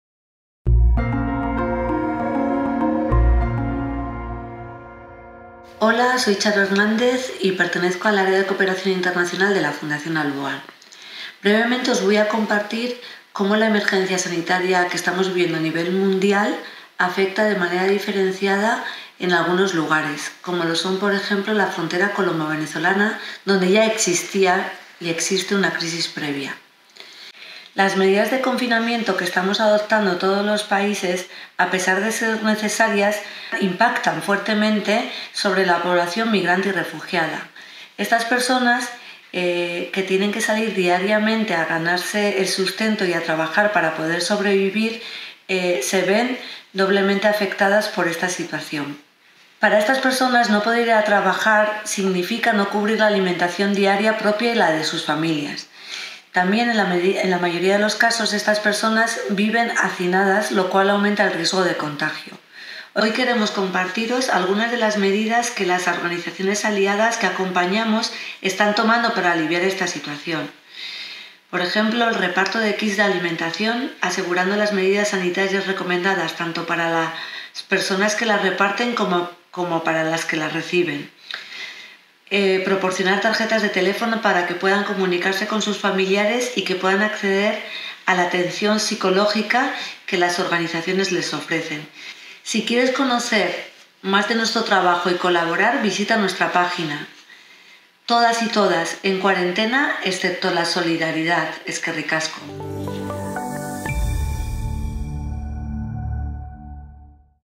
Ante la pandemia, Ciudadanía Global. Atención a migrantes en frontera colombo-venezolana
Ante la pandemia global del Covid19 estamos trabajando más que nunca para apoyar a las comunidades y personas que más lo necesitan. Nos preocupa la situación que estamos viviendo en nuestro entorno más cercano y en muchos lugares del mundo, donde las condiciones sociales y sanitarias son aún más adversas. En la frontera entre Venezuela y Colombia damos cobertura a la labor de atención a personas migrantes y refugiadas. Nos lo cuenta Txaro Hernandez, del Departamento de Cooperación de ALBOAN.
👉 Tú también puedes colaborar, apoyando la Emergencia Global